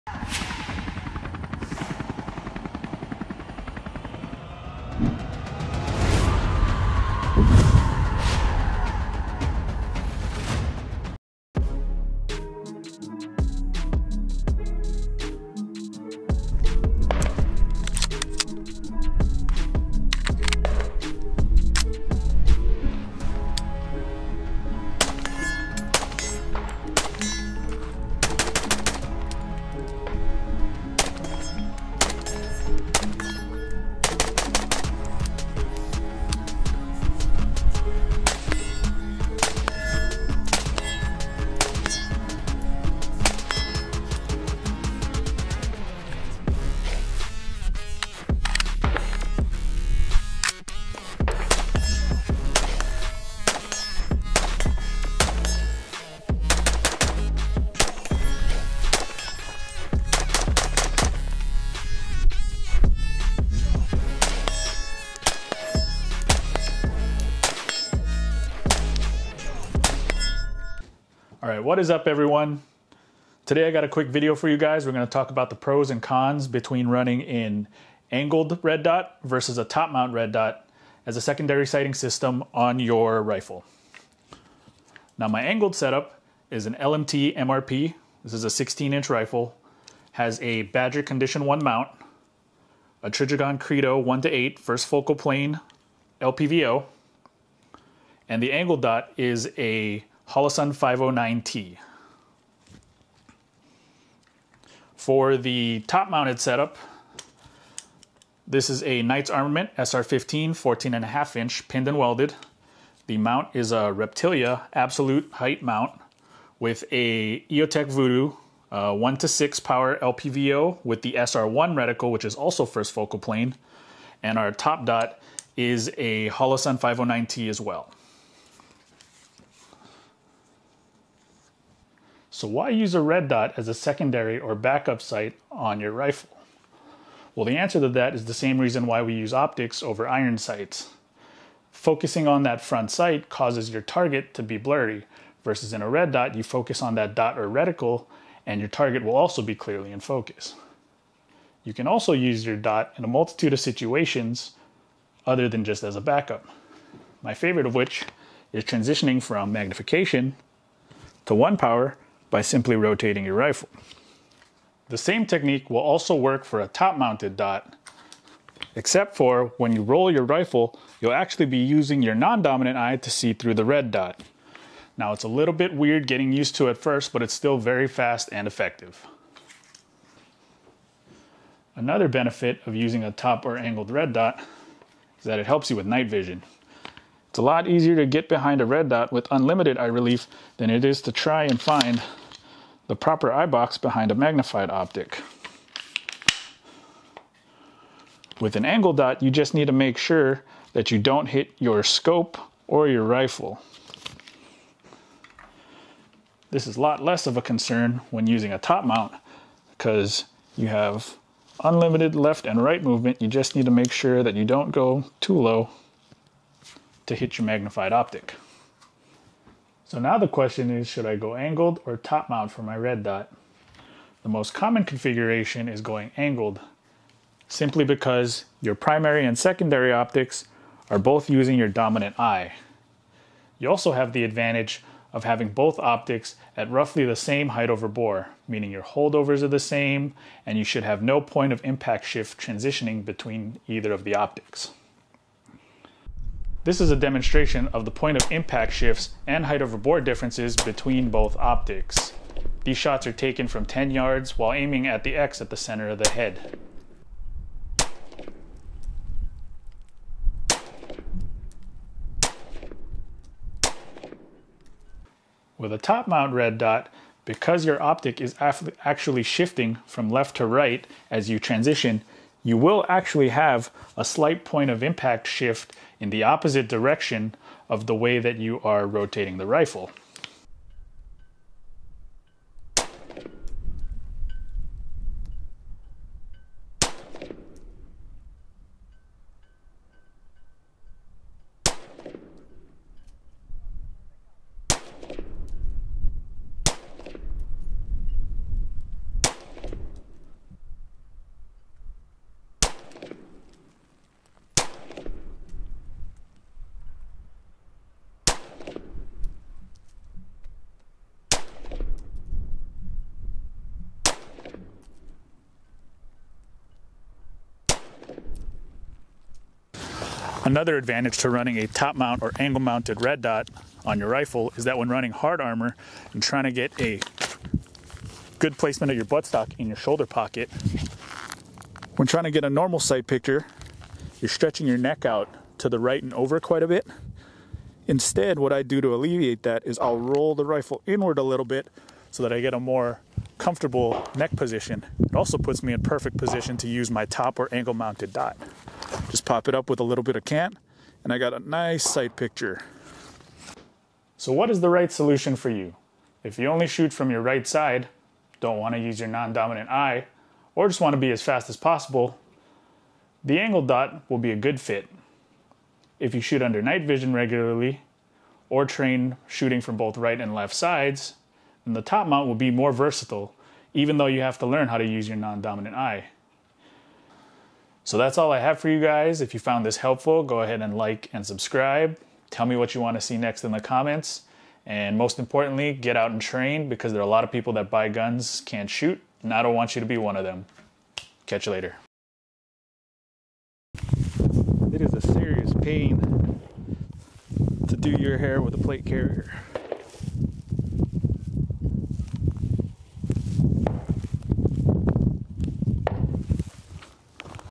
Top Mounted or Angled Mounted Red Dot? Adding a Secondary/Backup Optic to Your Rifle
Interested in adding a secondary/backup red dot optic to your recce/general purpose rifle? We discuss the pros and cons to using a top mounted dot vs an angled mounted dot and demonstrate how to properly use each.

Chapters:
0:01:24 Angled Mounted Red Dot Rifle Setup
0:01:44 Top Mounted Red Dot Rifle Setup
0:02:10 How to Use a Secondary/Backup Red Dot
0:03:39 Pros/Cons: Angled Dot vs Top Mounted Dot

Demo Rifles:
Top mounted dot: Knights Armament (KAC) SR-15 with Eotech VUDU 1-6x
Angle mounted dot: Lewis Machine and Tool (LMT) MLR MRP with Trijicon Credo 1-8x
*Both rifles are using a Holosun 509T red dot as the secondary optic.

Some Gear From this Video:
SKD Pig Gloves
Peltor Comtac Hearing Protection
Oakley SI Multicam Sunglasses
Crye Precision G4 Shirt and Pants (MC Tropic)
G-Code soft shell scorpion pouches